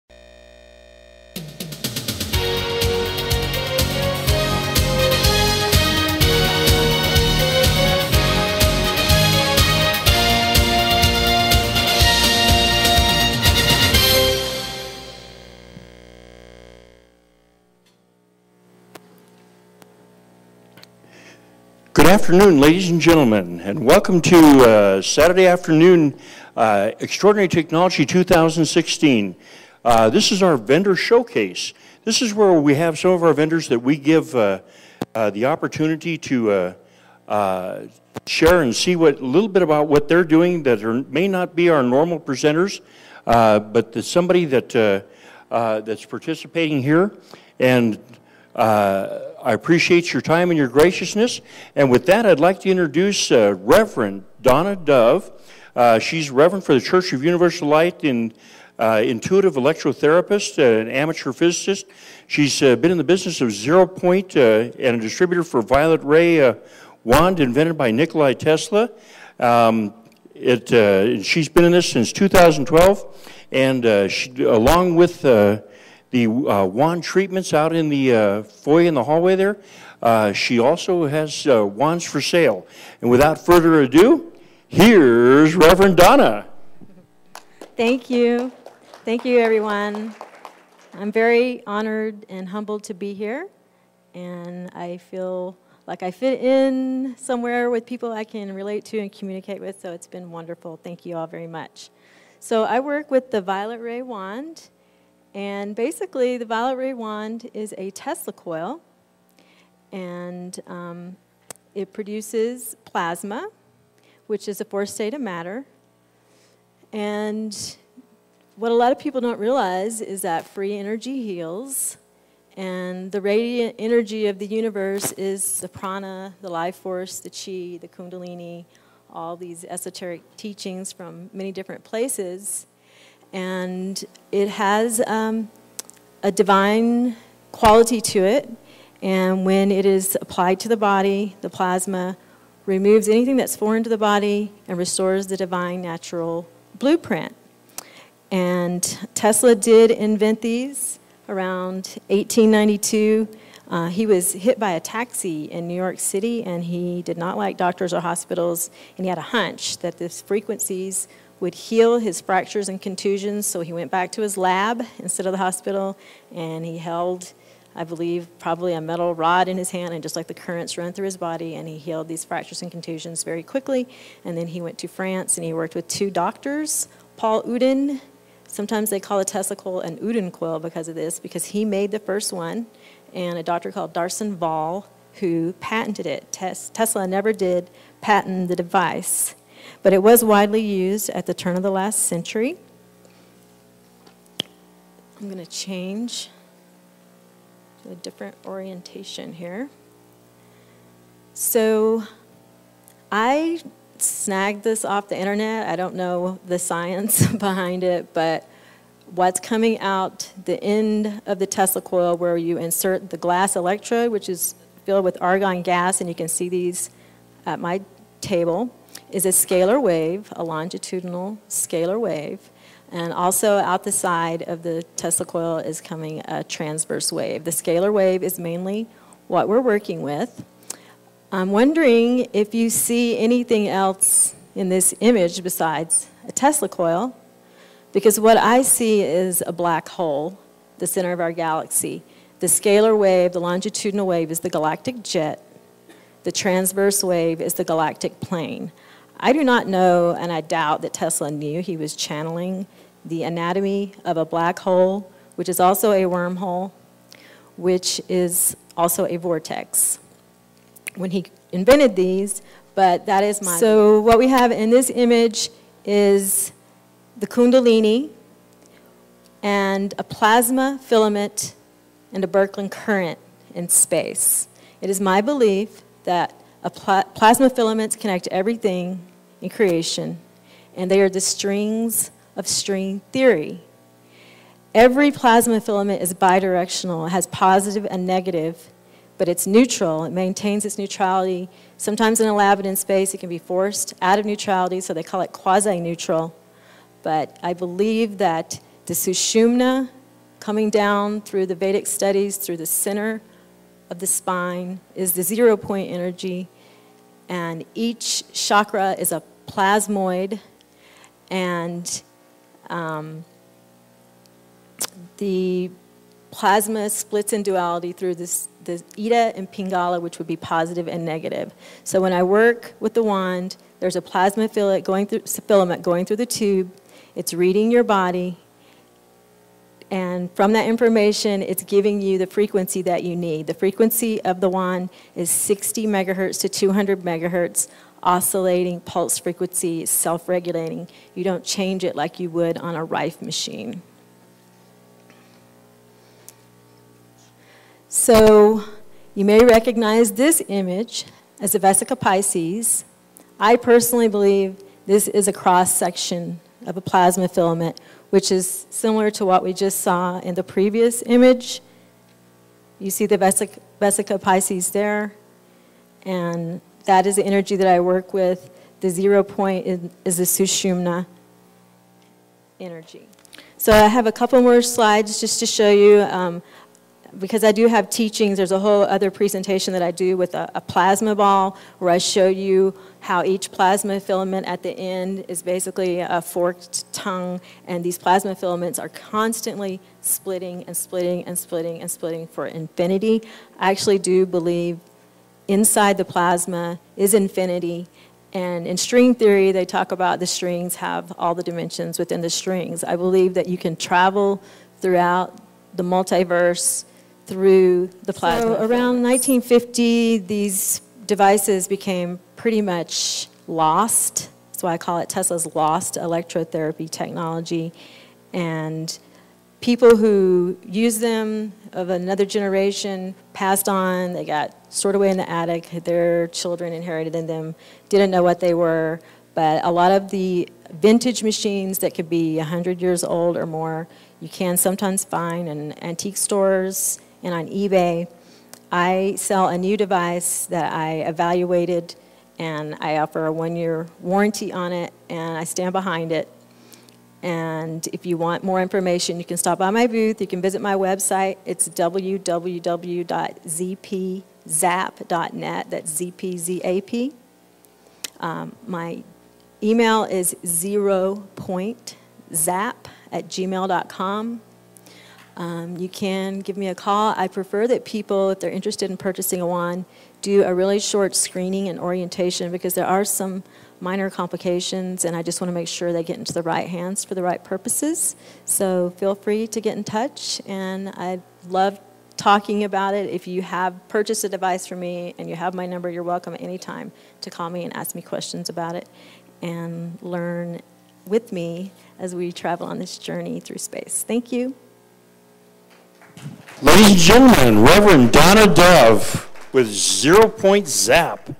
Tesla ExtraOrdinary Technology Conference 2016 Zero Point Zap Violet Ray Wands
Rev. Donna Dove presents the Violet Ray Wand and the divine nature of Plasma, the 4th state of matter at the 2016 Tesla ExtraOrdinary Technology Conference in Albuquerque, NM during the Vendor Showcase. For more information on the Violet Ray Wand or to purchase and for more information on the Conference